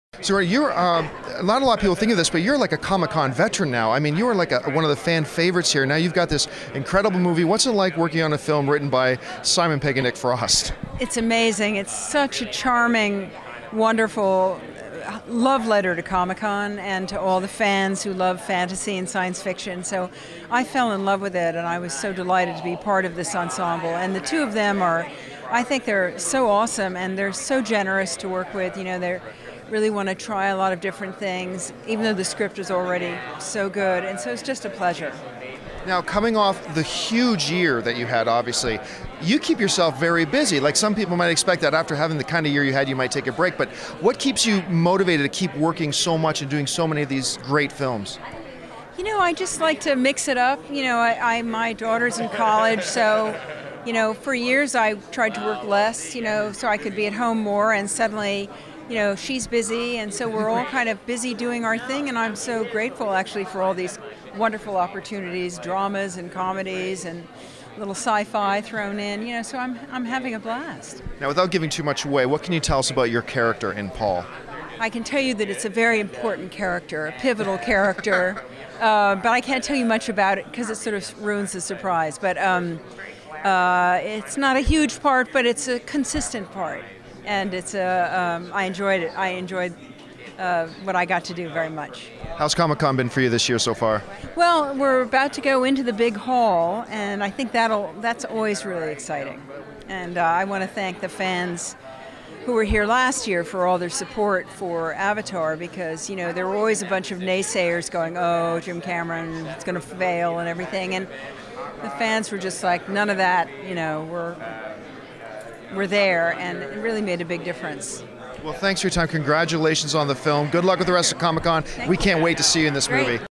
Sigourney Weaver Talks "Paul" with AMC
Actress Sigourney Weaver talks with AMC Script-To-Screen at the recent San Diego Comic Con about her new film "Paul"